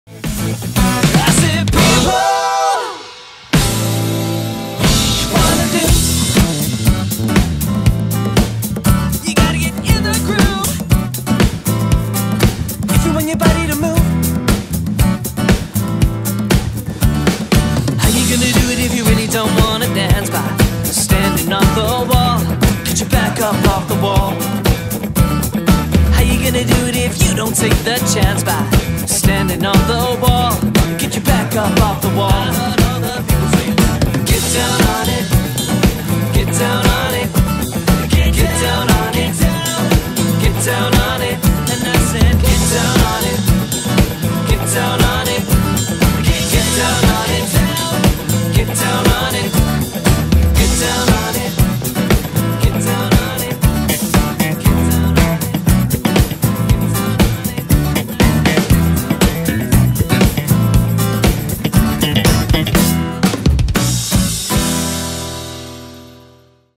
Walk The Aisle Showreel - AliveNetwork.com
Covering the biggest and best hits from Harry Styles, Stevie Wonder, Oasis, Lizzo, Elvis and tons more, Walk The Aisle are a party-starting trio you'll absolutely love! This professional band know how to create an unforgettable celebration with their exciting live sets and optional extras such as Ibiza Sax and DJ sets, Acoustic Sets, Sparks Machine and more!